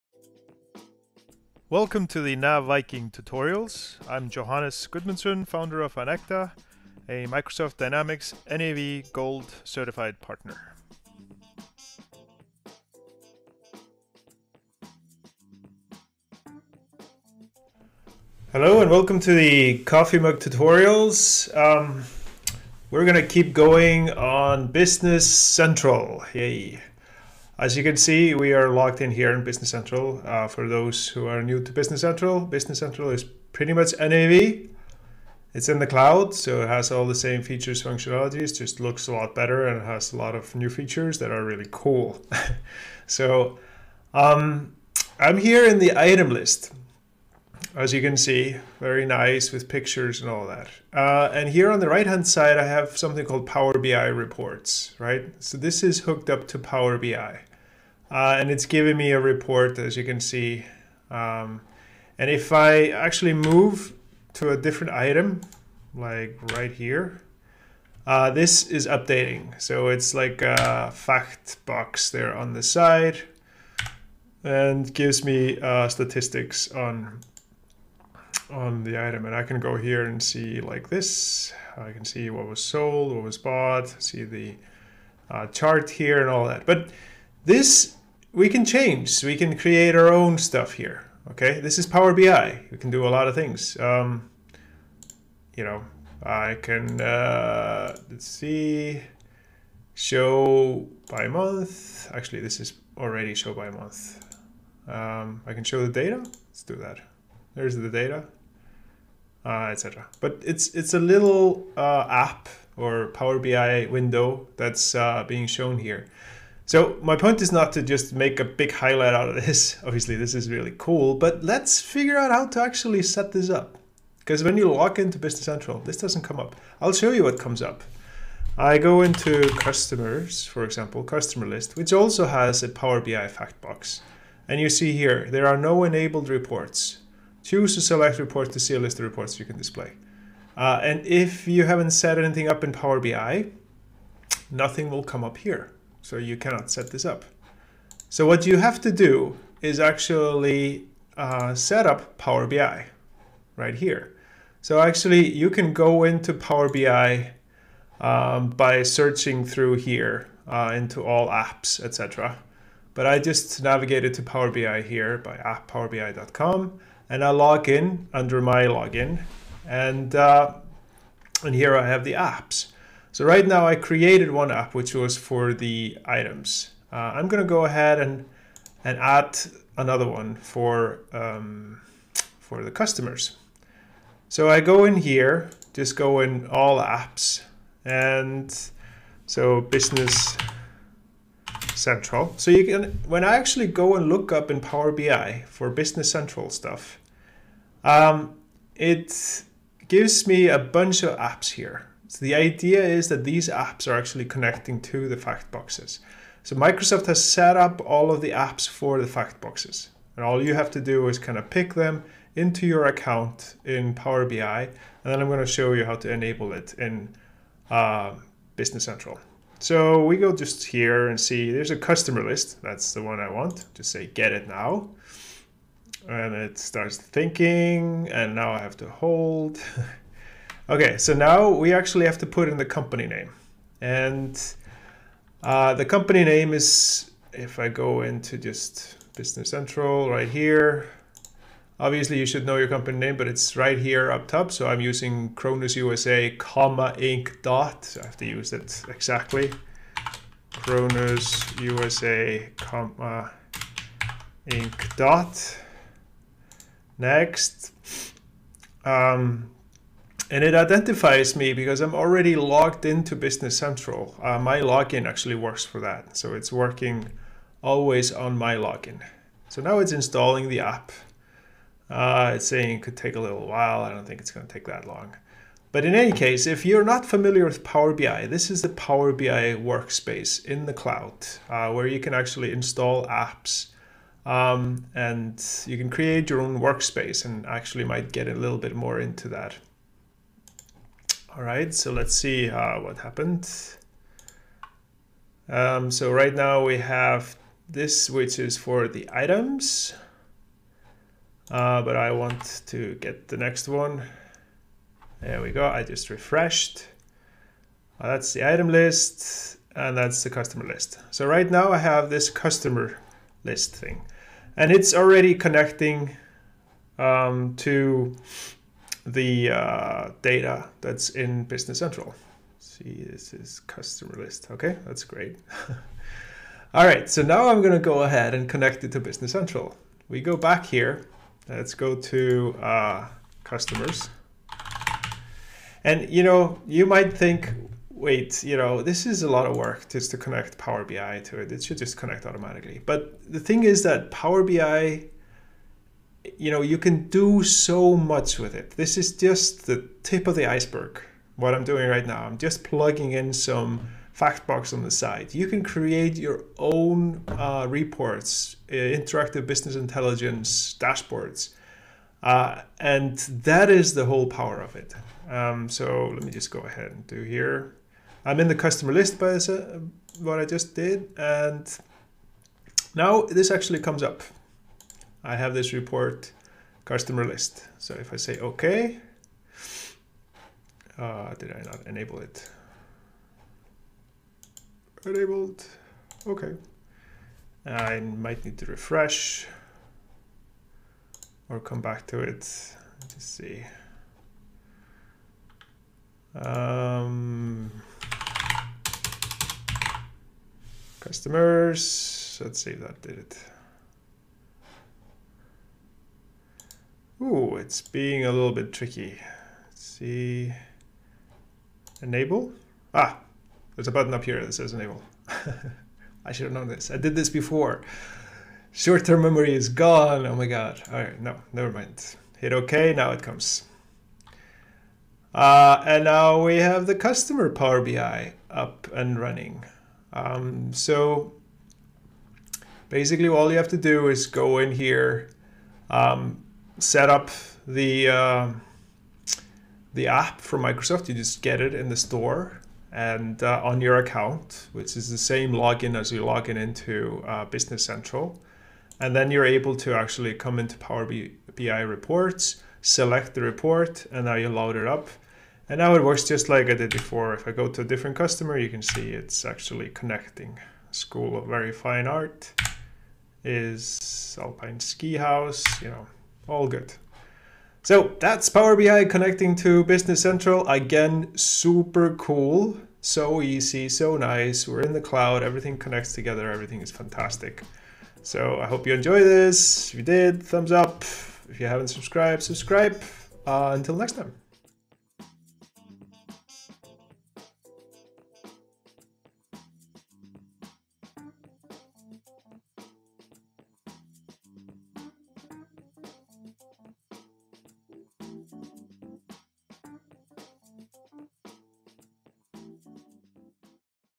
Business Central: Setting up Power BI
Learn how to set up Power BI in Business Central in Microsoft Dynamics 365 Business Central with Johannes Gudmundsson in this coffee mug tutorial.